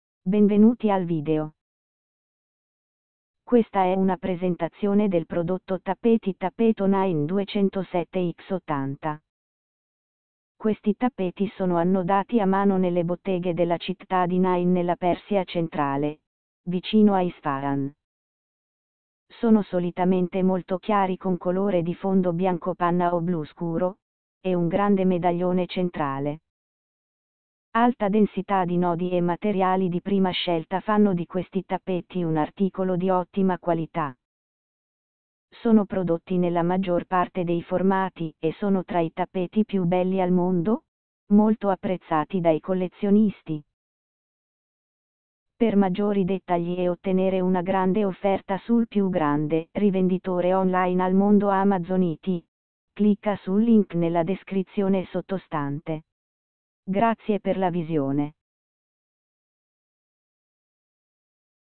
Tappeto Nain 207x80
Questi tappeti sono annodati a mano nelle botteghe della città di Nain nella Persia centrale, vicino a Isfahan. Sono solitamente molto chiari con colore di fondo bianco panna o blu scuro, e un grande medaglione centrale. Alta densità di nodi e materiali di prima scelta fanno di questi tappeti un articolo di ottima qualità. Sono prodotti nella maggior parte dei formati e sono tra i tappeti più belli al mondo, molto apprezzati dai collezionisti.